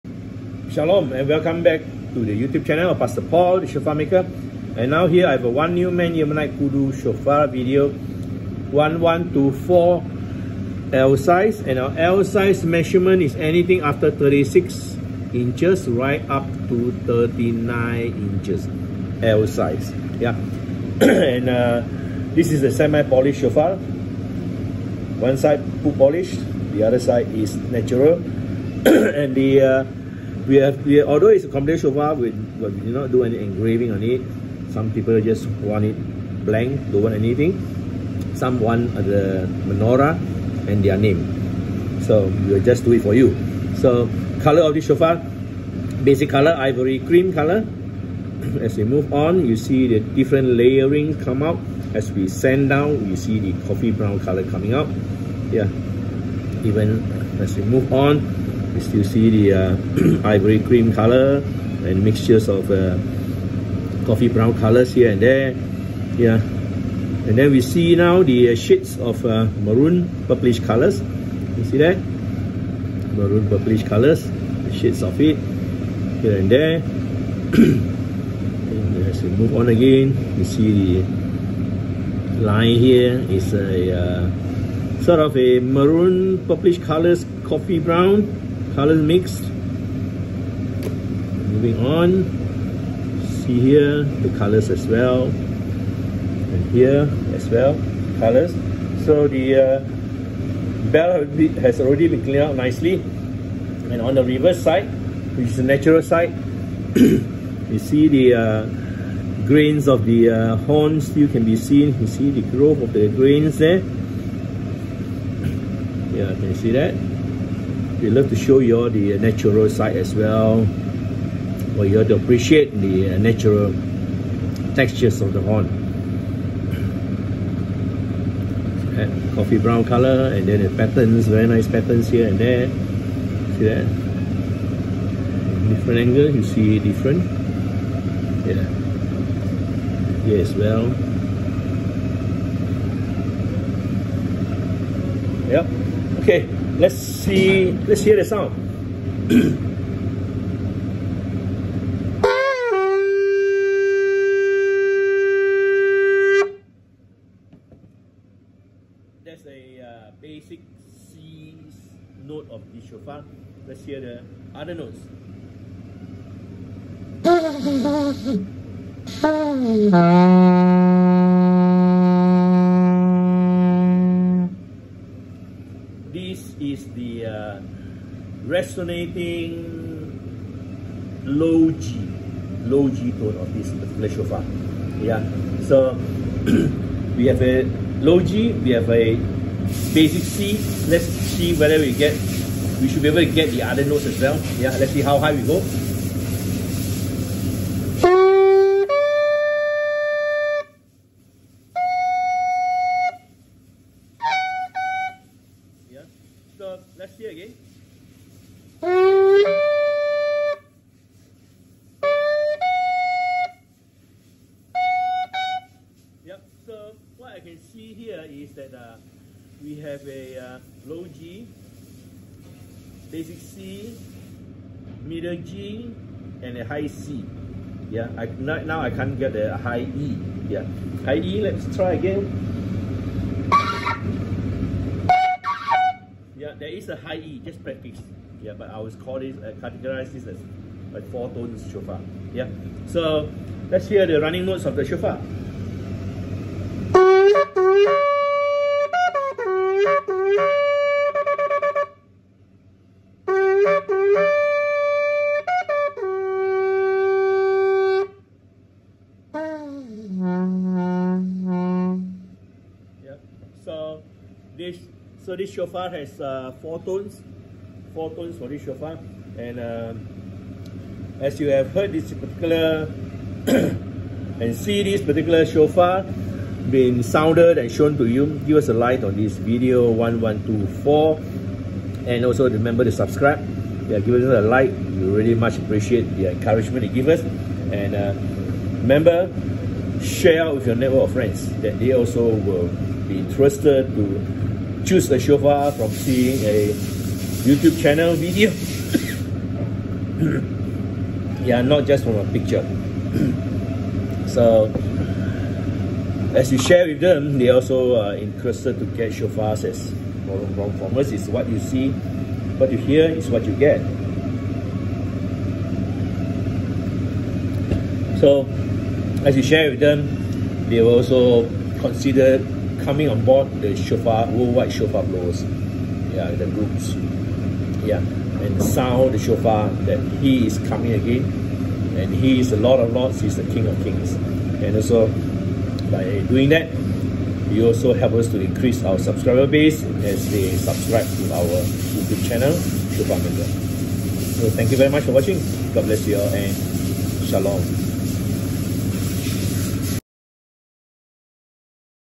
SOLD •ShofarMaker v1124_L. 4 Tone ONM Yemenite Kudu Shofar. Semi Polished.
A 4 Tone Commanding Strong Vibrant Fine Tuned Shofar with a low G sounding.
A Semi Polished shofar with one side maintained natural with it grooves, knuckles and battle scars to showcase  the Kudu Horn cleaned out and in its textured natural state.
Olive Cream with Coffee Brown & Maroon Purplish colours.
View & Listen to Appreciate.
Tq for your support & Shalom.
Ybu.